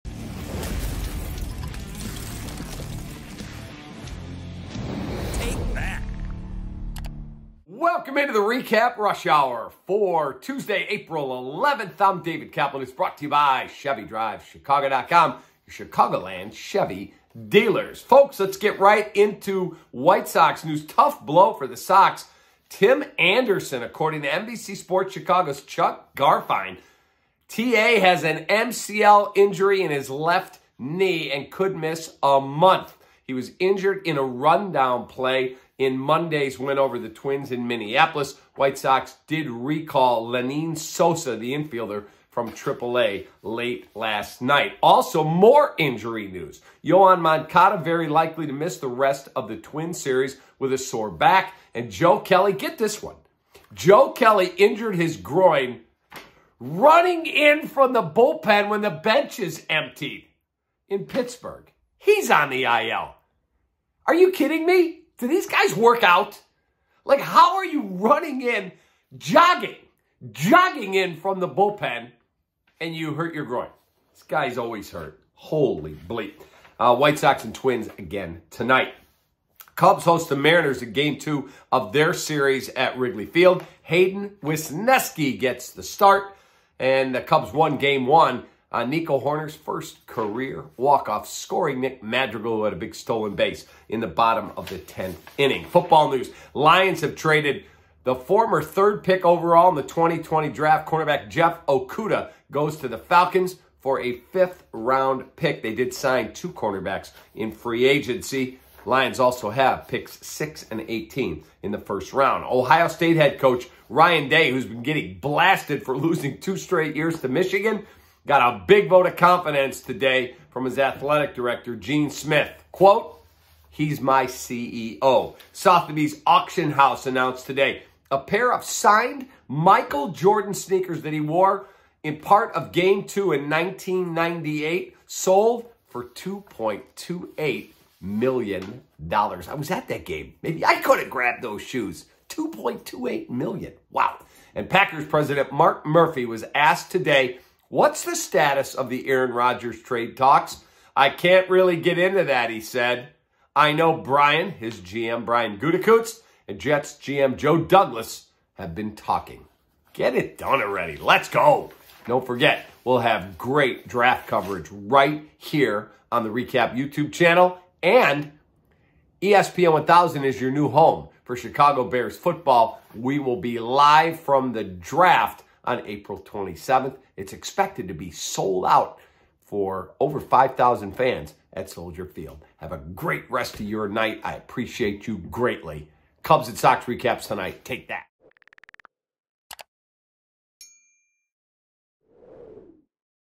REKAP Rush Hour 🚗 - Chicago White Sox star Tim Anderson headlines injury news, Aaron Rodgers update
The White Sox are 11 games in and riddled with injury news already. Tim Anderson is going to be out up to a month with an MCL sprain after a mixup on a rundown last night. It’s a major blow to the Sox who are already without Eloy Jimenez, and Yoan Moncada, who may miss the remainder of the Twins series with a bad back.

The Cubs send Hayden Wesneski to the mound against Seattle.

The Lions shipped for #3 overall pick Jeff Okudah to the Falcons. Packers President Mark Murphy says in regards to Aaron Rodgers that the two sides ‘remain talking’ discussing a trade to the Jets.

And a piece of a pair of shoes Michael Jordan wore in the 1998 NBA Finals sold for over $2 million dollars today.

➡️  Get a FREE SEO Audit

📖 Bernie's Book Bank (Distributed Over 22 million Books since 2012!)